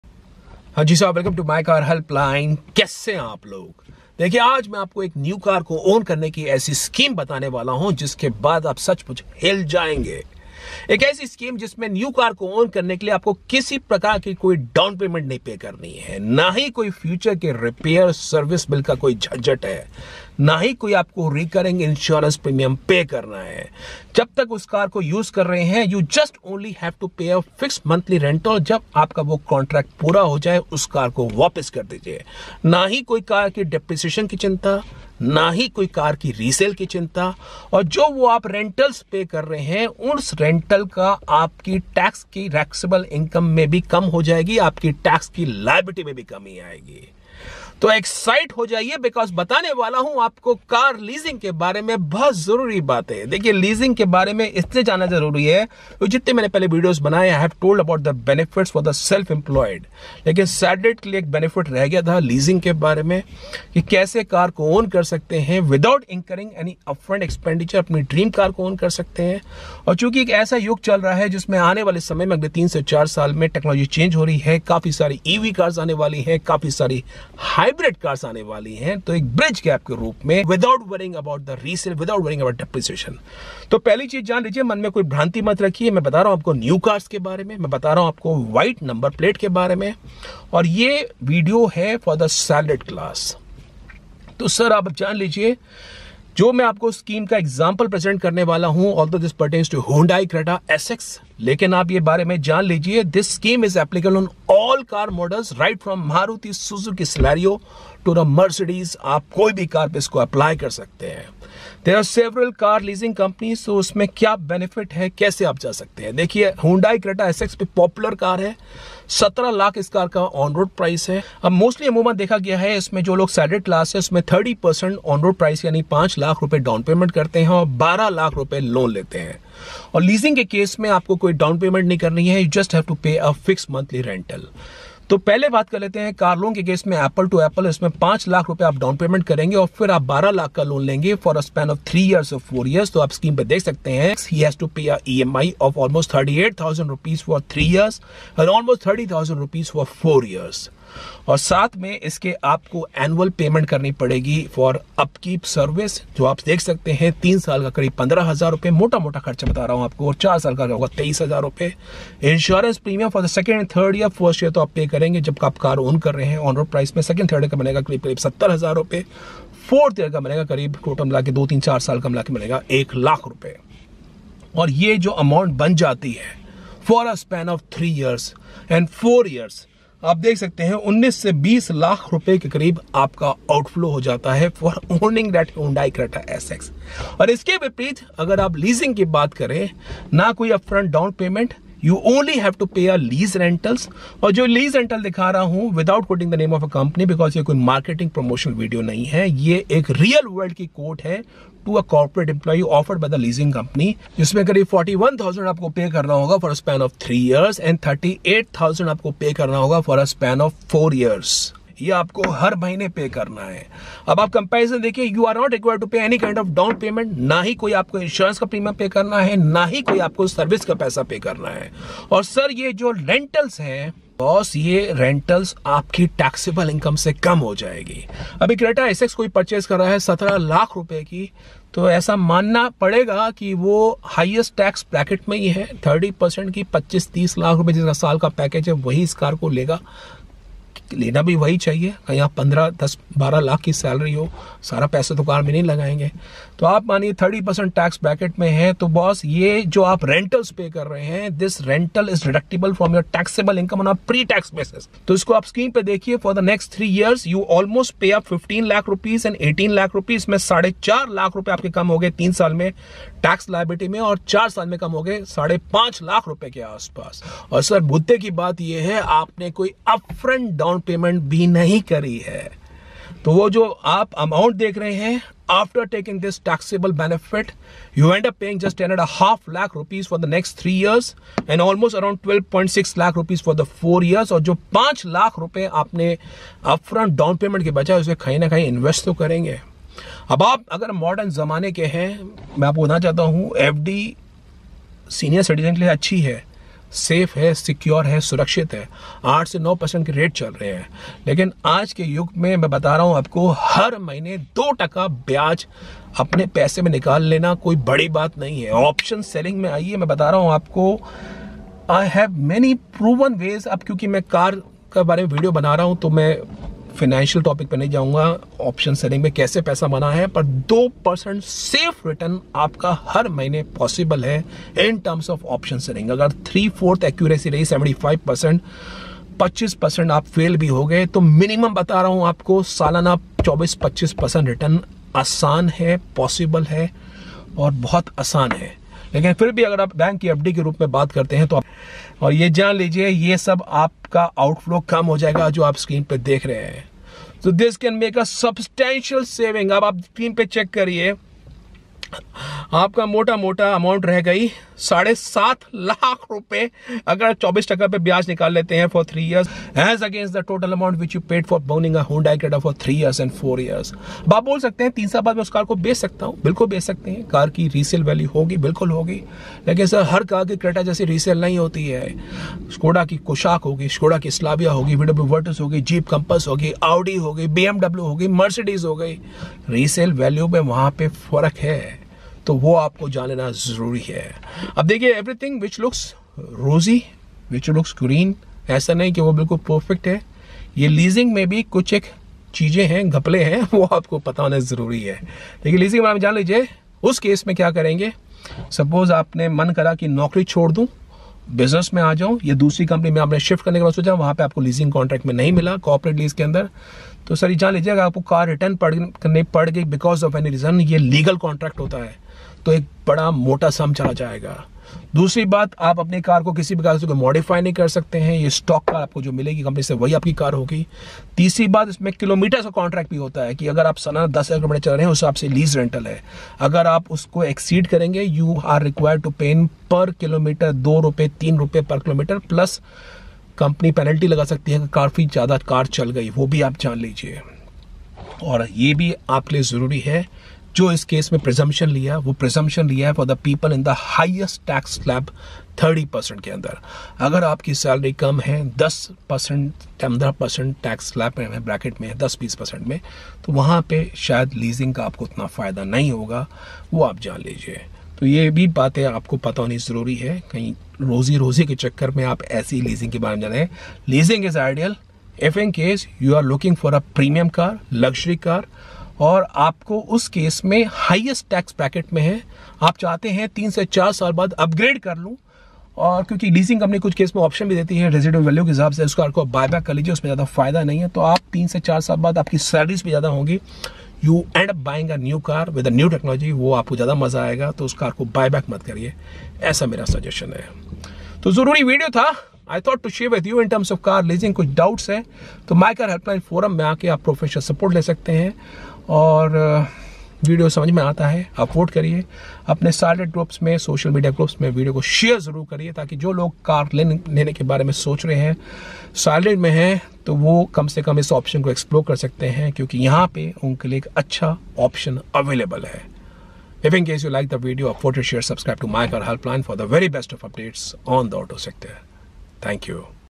IS LEASING BETTER AGAINST BUYING CAR ? CAR LEASE Vs. LOAN MATHS
Considering buying New Car, now no need to buy your Dream SUV at exorbitant prices, here is Car Lease Explained in comparison to traditional route of buying car or on Loan. Real Life Example on Car Lease with Tax Implications shared for Salaried Class